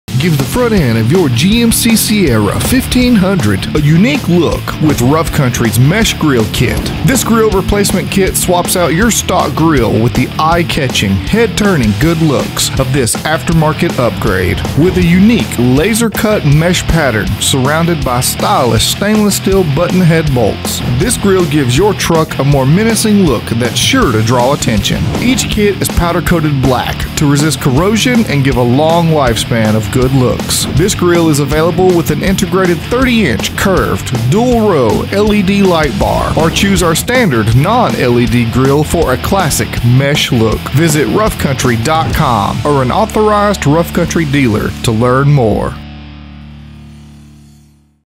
2016 - 2018 GMC Sierra 1500 Mesh Grille Kit by Rough Country
Give the front-end of your GMC Sierra 1500 a unique look with Rough Country’s Mesh Grille kit! This grille replacement kit swaps out your stock grille with the eye-catching, head-turning good looks of this aftermarket upgrade. With a unique, laser-cut mesh pattern surrounded by stylish, stainless steel button head bolts, this grille gives your truck a more menacing look that’s sure to draw attention. Each kit is powder-coated black to resist corrosion and give a long lifespan of good looks.

This grille is available with integrated 30-inch curved dual-row LED light bar or choose our standard, non-LED grille for a classic mesh look. Includes a mountable on/off switch, wiring harness, and our 3 Year Warranty!

Visit roughcountry.com or an authorized Rough Country dealer to learn more.